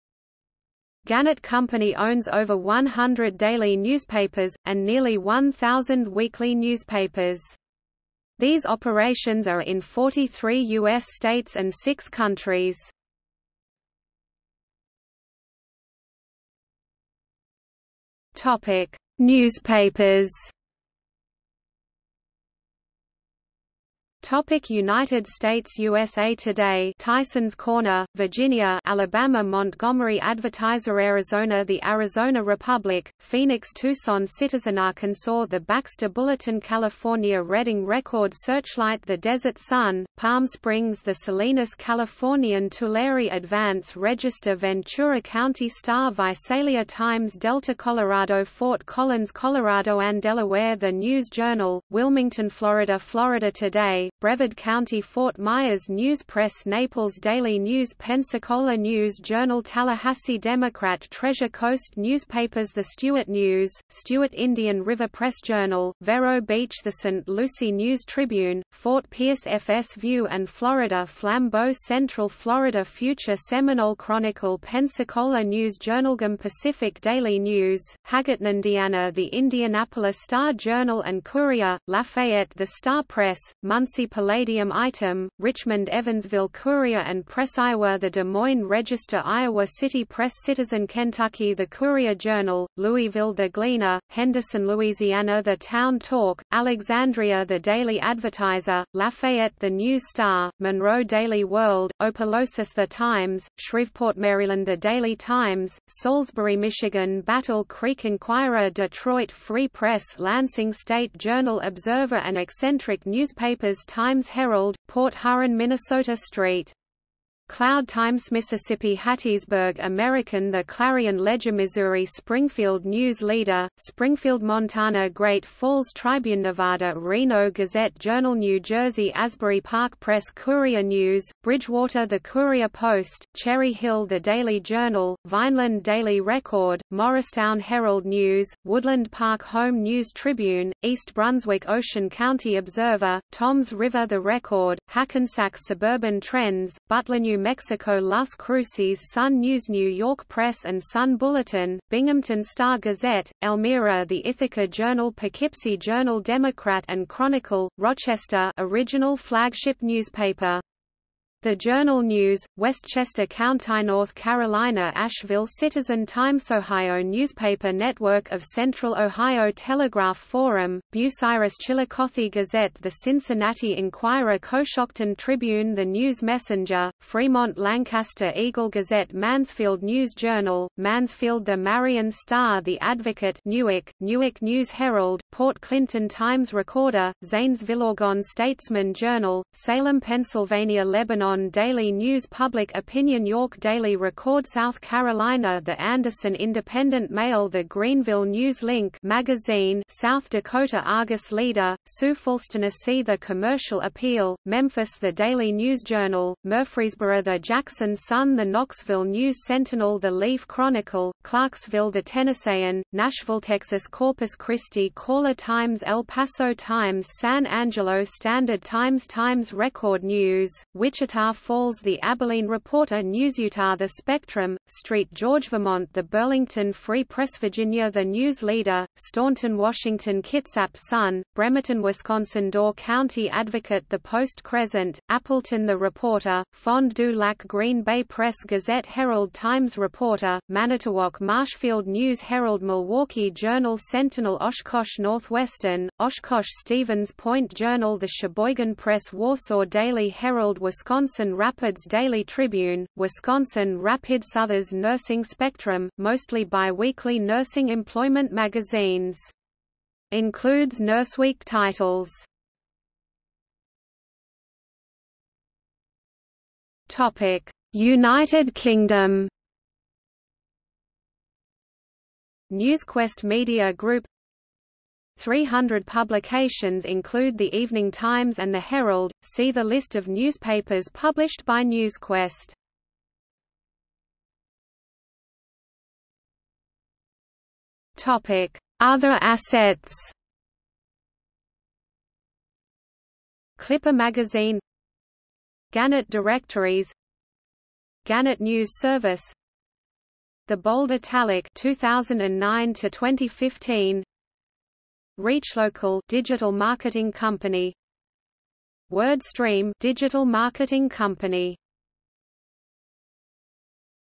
List of assets owned by Gannett Company | Wikipedia audio article
00:00:14 1 Newspapers
00:00:24 1.1 United States
00:04:23 1.2 United Kingdom
00:04:44 2 Other assets

- Socrates

SUMMARY
Gannett Company owns over 100 daily newspapers, and nearly 1,000 weekly newspapers. These operations are in 43 U.S. states and six countries.